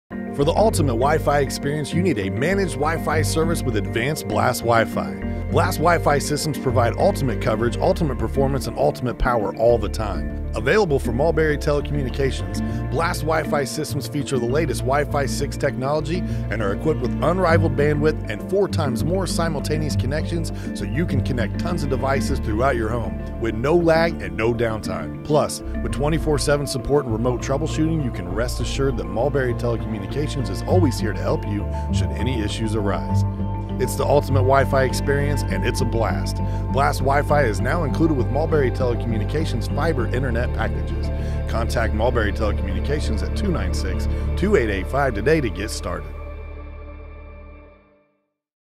Ultimate WiFi from Mulberry Telecommunications
GigaSpire Wireless Routers and Ultimate WiFi are now included with all Mulberry Telecommunications Fiber Optic Internet Packages.